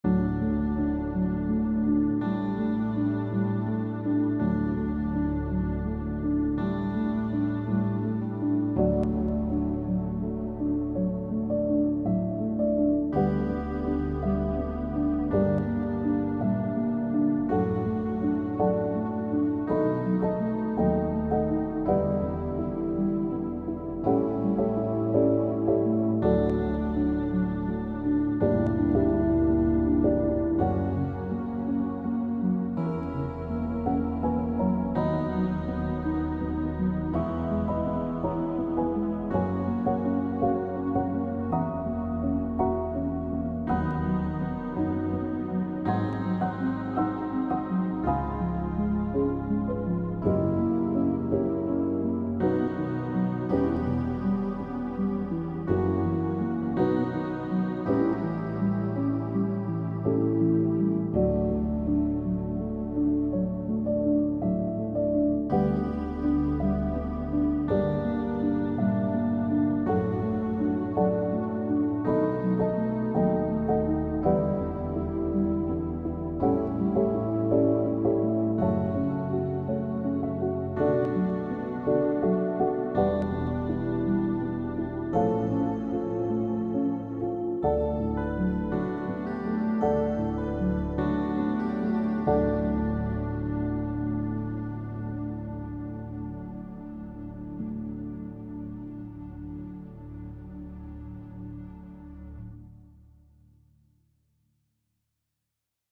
Nadia's Cozy Dream
Nadia is cozy, she must be having a nice dream...
I couldn't fix those clicks... in volume..